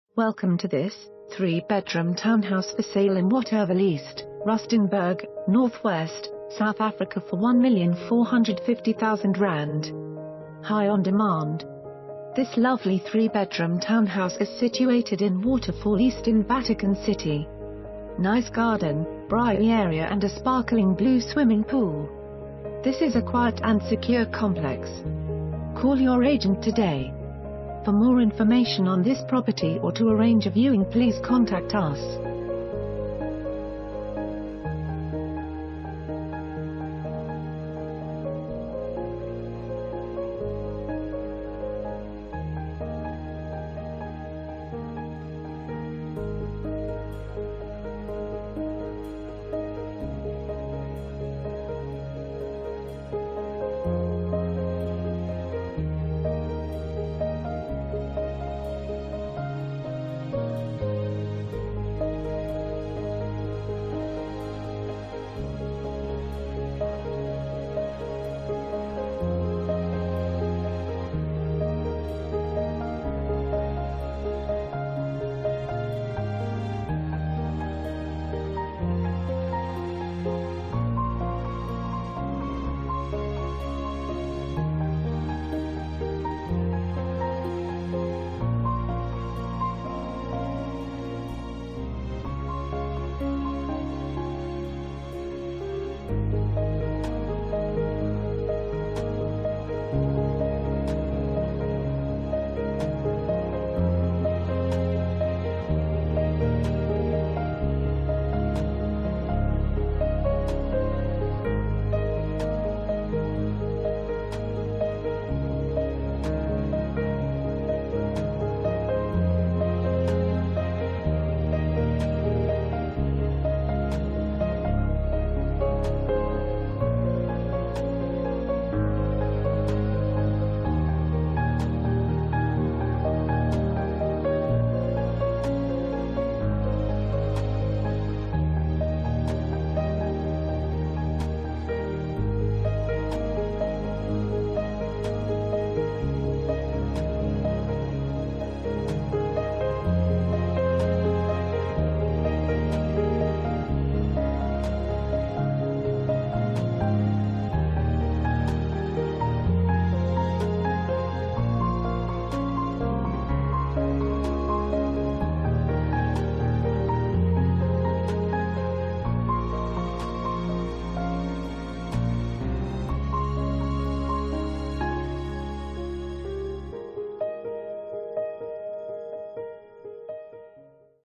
3 Bedroom Townhouse For Sale in Waterval East, Rustenburg, North West, South Africa for ZAR 1,450...
HIGH ON DEMAND!This lovely 3 bedroom townhouse is situated in Waterfall East In Vatican City!Nice garden, braai area and a sparkling blue swimming pool!This is a quiet and secure complex!Call your agent today!